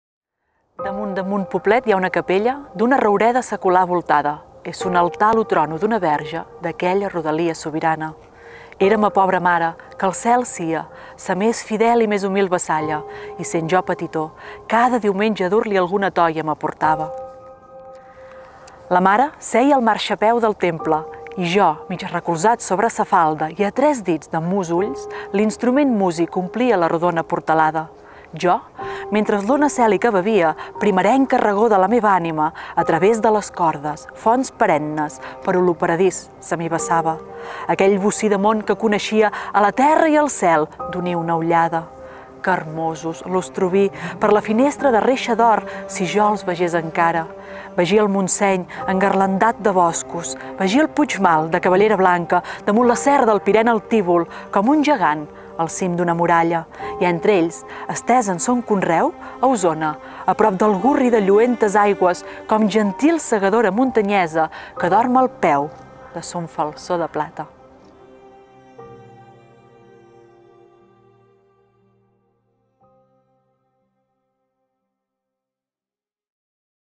Ruta Verdaguer a Folgueroles: 'L'arpa'
En aquest tast d'una RutaVerdaguer les guies us expliquem els llocs verdaguerians de Folgueroles. Llocs i mots arrelats a la infantesa de Jacint Verdaguer i a l'imaginari dins de la seva obra. Si visiteu la Casa Museu Verdaguer no us perdeu la part indispensable de les rutes literàries.